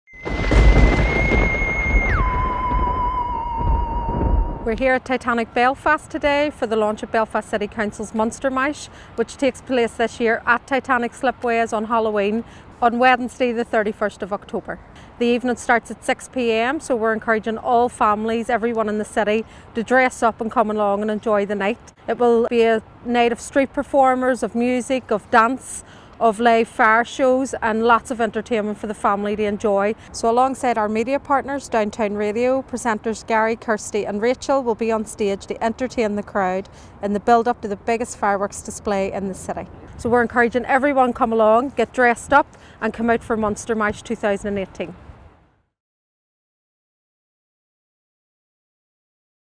Fangtastic Halloween Monster Mash returns to Belfast on Halloween night!
The Halloween Monster Mash returns to Titanic Slipways on Halloween Night (Wednesday 31 October) - promising all a ‘ghoulishly’ good time! Lord Mayor of Belfast, Councillor Deirdre Hargey was on hand to launch the Spooktacular event. October 2018